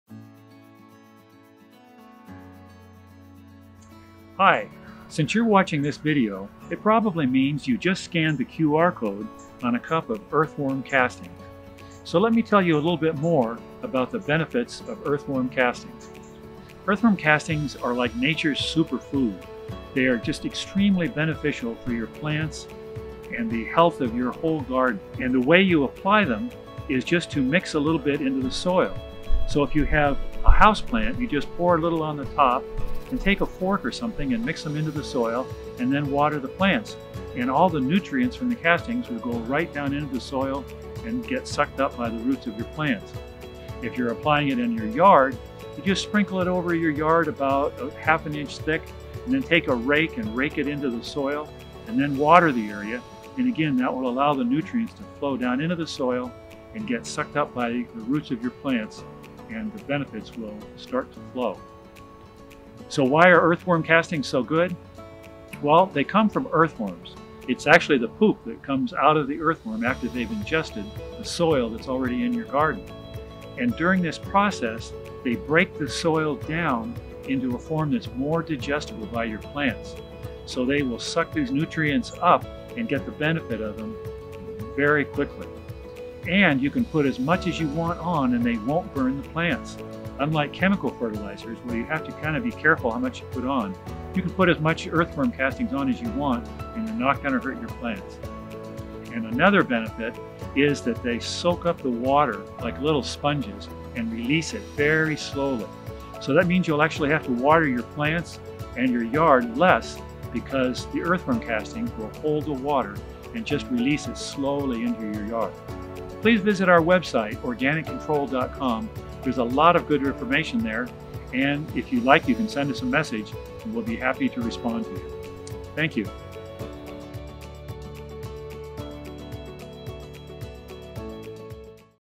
How to Use Earthworms Castings
Earthworm Castings are nature's all-natural superfood for plants. Organic Control Founder Steve Hazzard explains how to to use earthworm castings for your houseplants and in your garden.